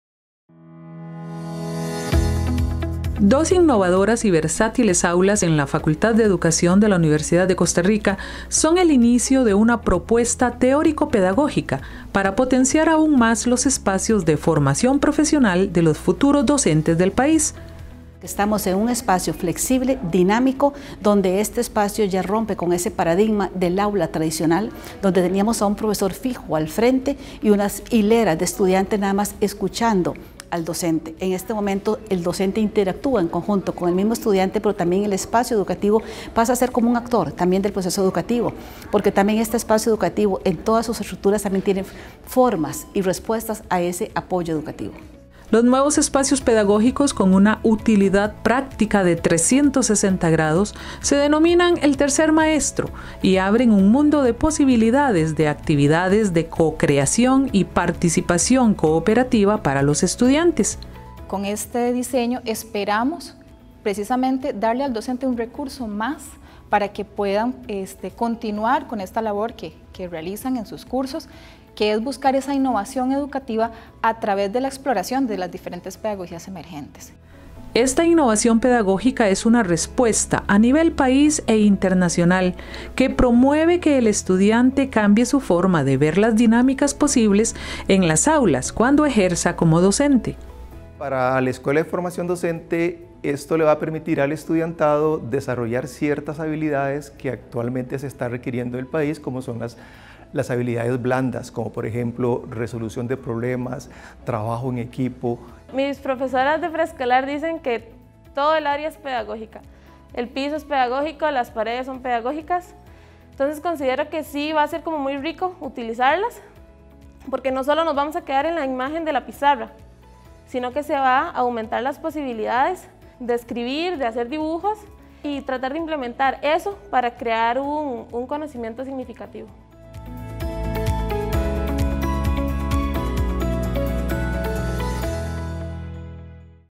El proyecto "Reorganización de los espacios educativos" inauguró sus dos primeras aulas
El Proyecto “Reorganización de los espacios educativos” inauguró en la Facultad de Educación sus dos primeras aulas.
Una innovadora y propositiva visión de formación profesional en educación como respuesta a las nuevas formas emergentes de los procesos educativos en la modernidad.